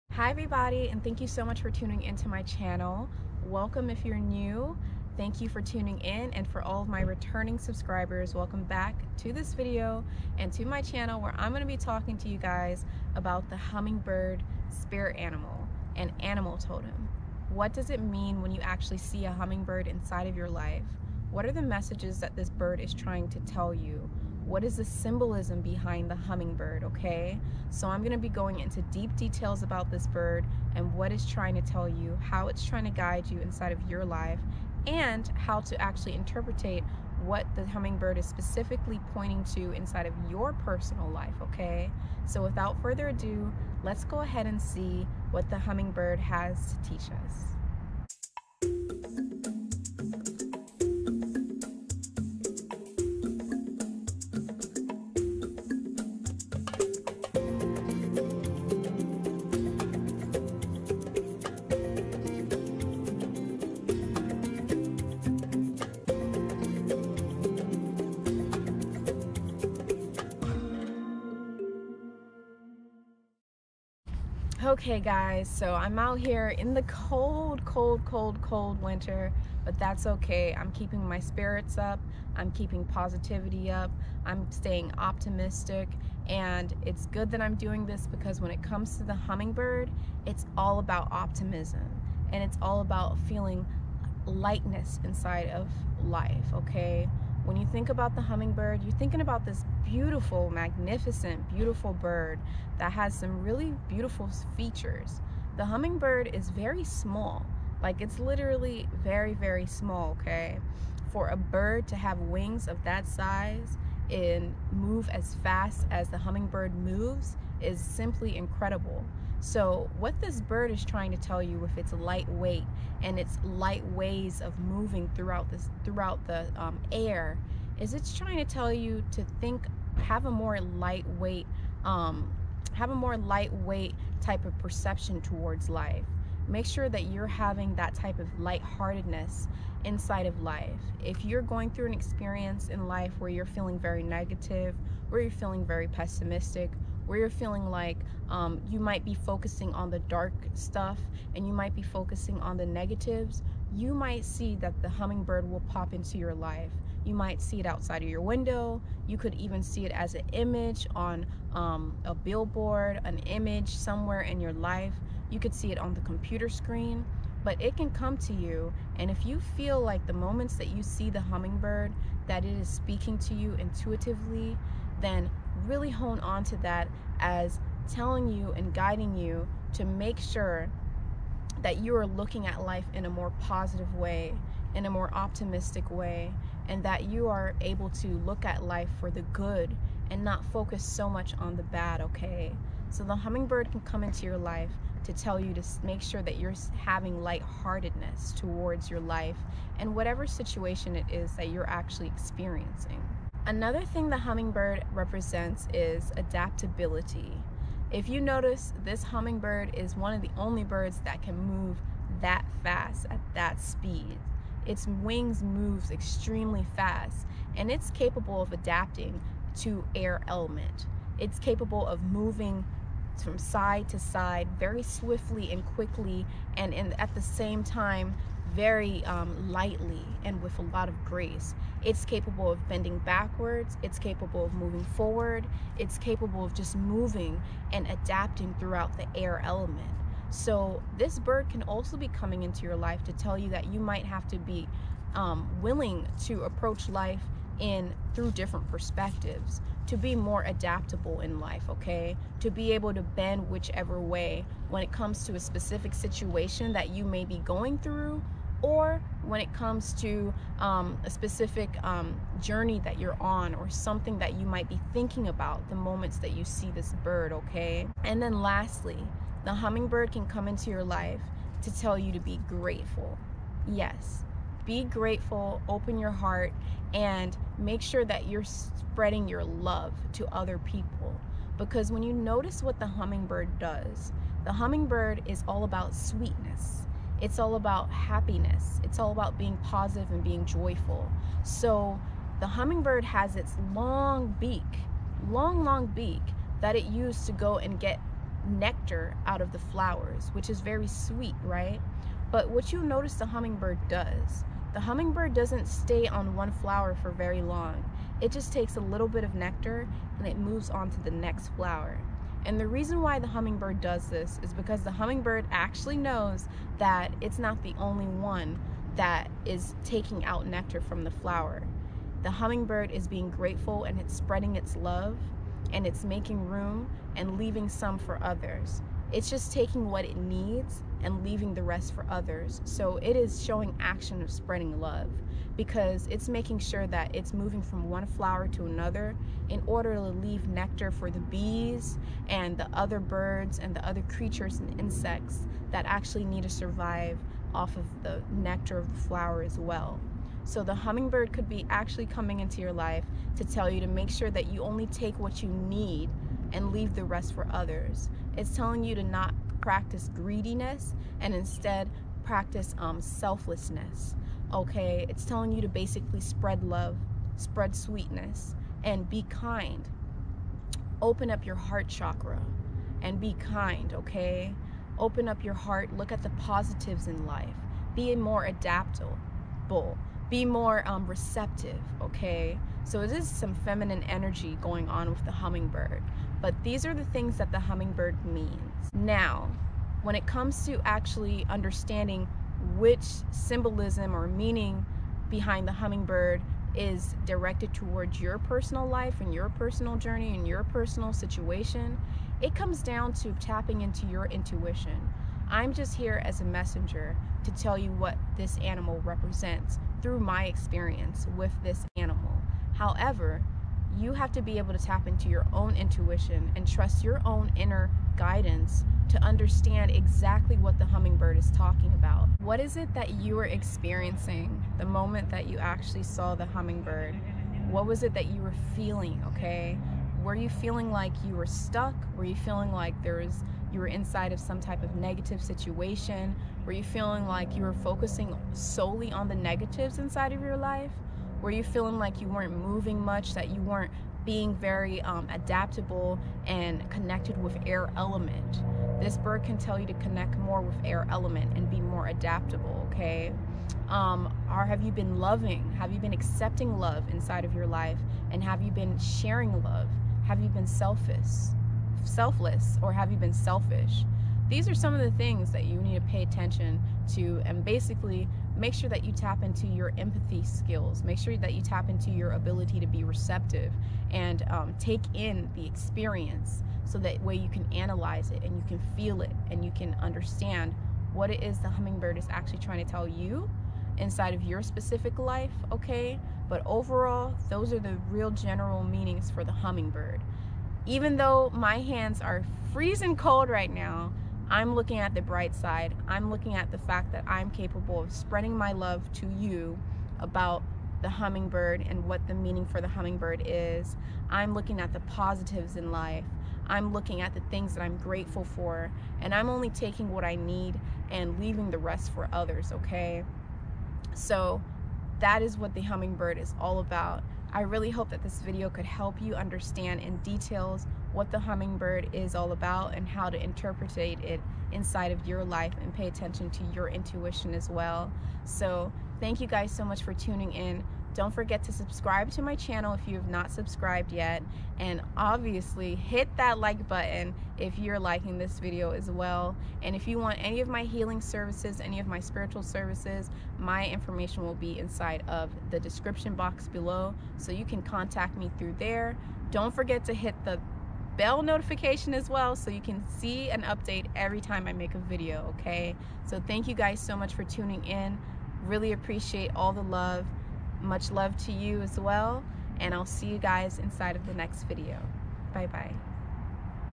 Hummingbird Spirit Animal Totem | Look Towards The Light
✨ Thank you for tuning into this video about the Hummingbird and what messages the Hummingbird has for you! Hummingbirds can come into our lives to tell us to express love and kindness. Have a more of a lighthearted approach to life; Think, move and act lightly. Just like the Hummingbird, now is a time to experience joy and love vibration.

✨ If you are interested in a personal Intuitive card reading,
Or any of my healing services

Bloomingdale Mack 😊💙💙